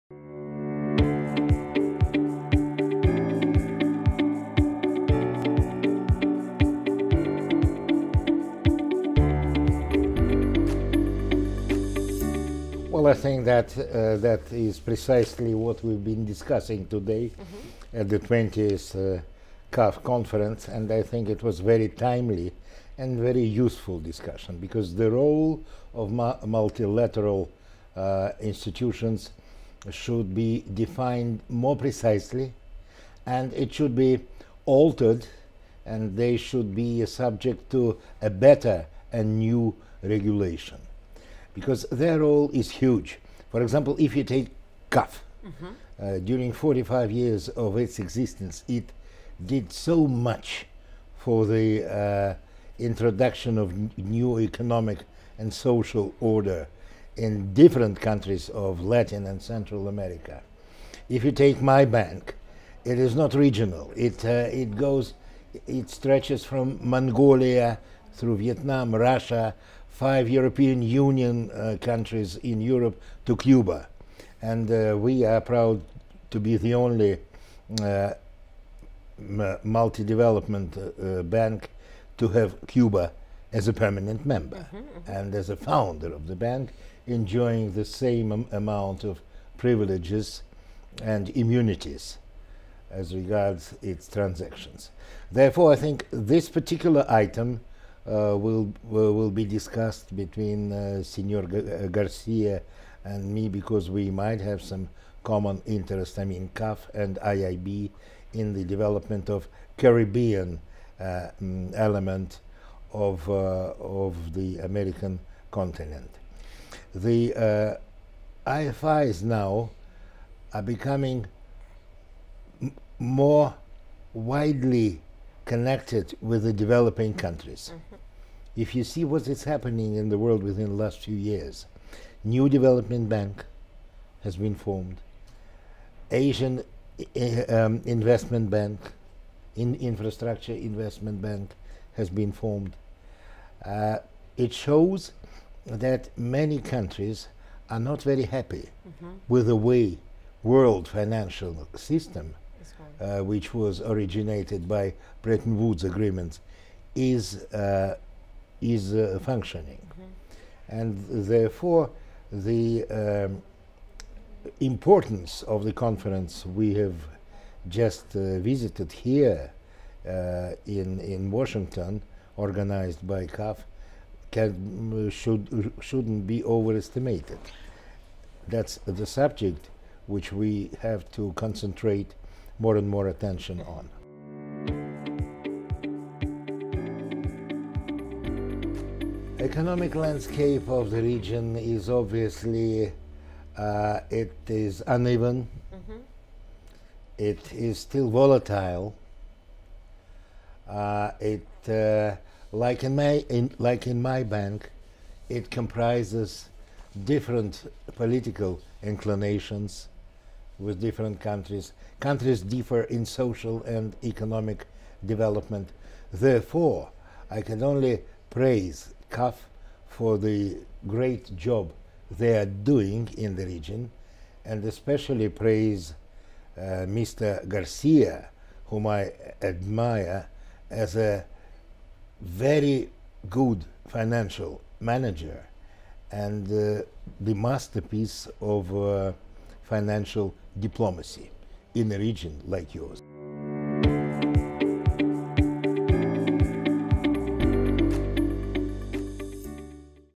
Nikolay Kosov: The importance of multilateral investment banks for Latin America.
Nikolay Kosov, Chairman, International Investment Bank